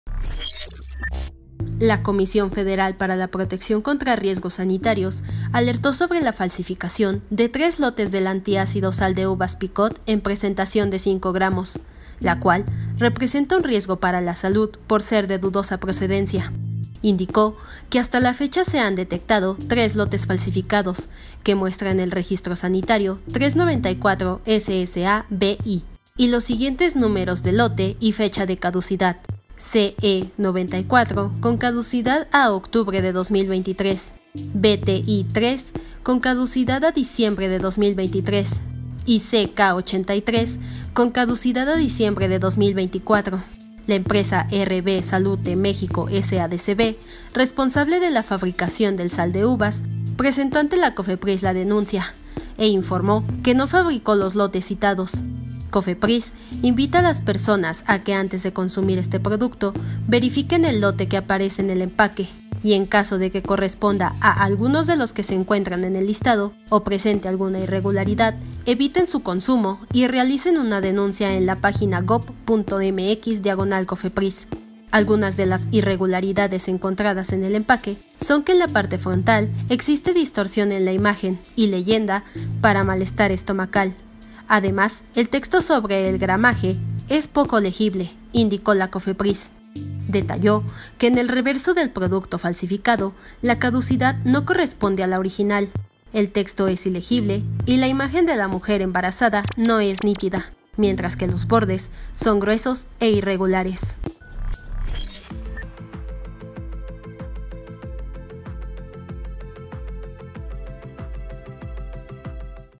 Cofepris alerta por falsificación de antiácido Sal de Uvas; así puedes identificar si es falso
A dos días de la cena de fin de año, la Comisión Federal para la Protección contra Riesgos Sanitarios alertó sobre la falsificación de tres lotes del antiácido Sal de Uvas Picot, en presentación de 5 gramos